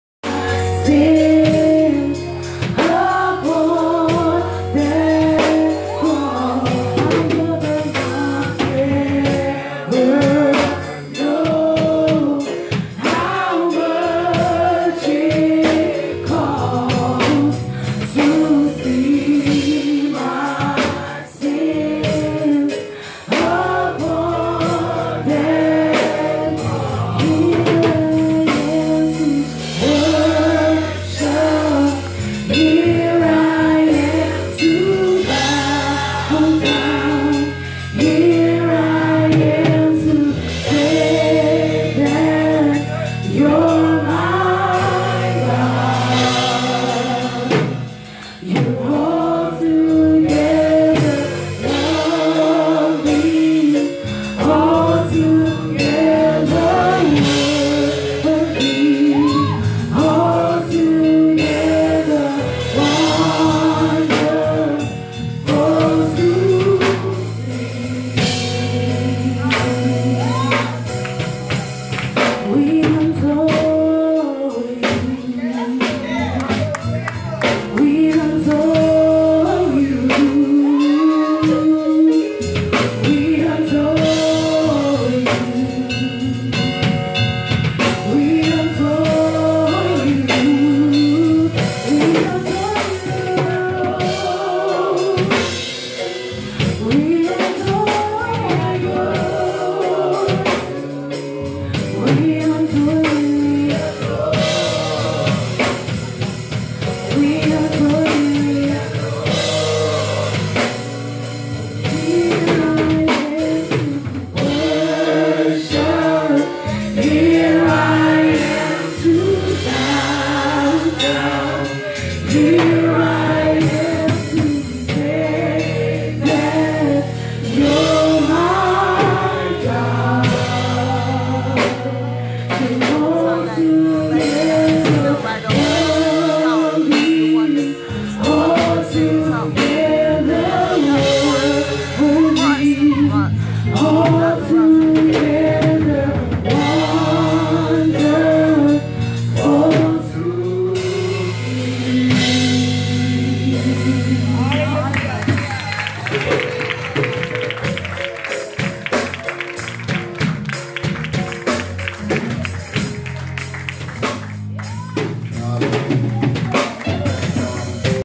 Solid Rock New Vision T.H.C.A.F: GLO Praise Team singing 9-13-2019 (Different Camera View)
Shontaye Michelle record this video same time i were record on my phone.